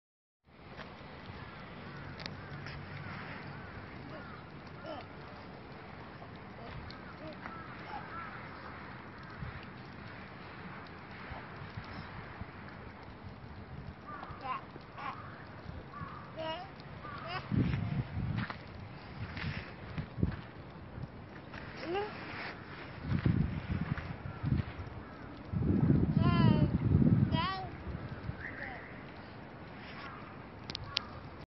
เล่นชิงช้า กับ น้อง อเล็กซานเดอร์ ไหม ครับ👼👼❤❤
ปาป๊า เป็น คน ถ่าย วีดีโอ